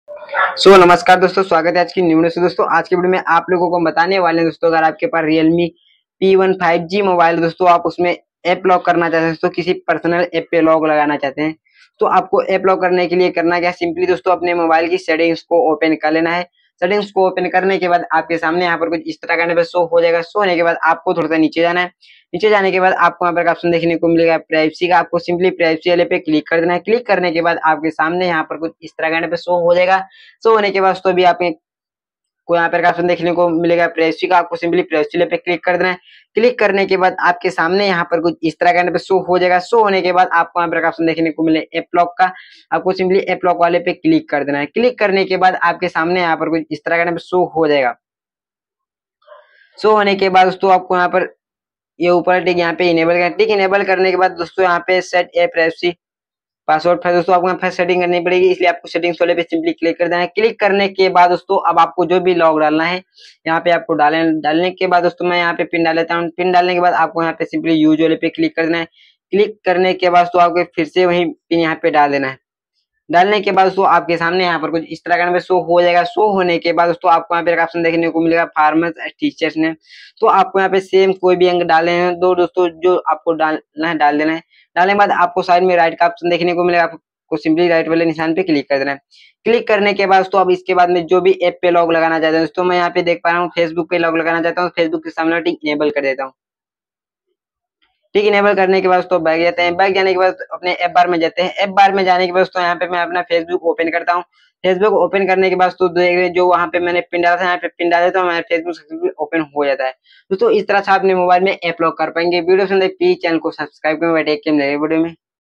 Realme p1 5g me app lock kaise lagaye/how to app lock in realme p1 5g setting/app lock realme p1 5g
Realme p1 5g me app lock kaise laga

Hello friends Aaj ki is video ke madhyam se maine aap logo ko bataya Realme p1 5g mobile me app lock kaise lagaye setting

display fingerprint lock in realme p1 5g mobile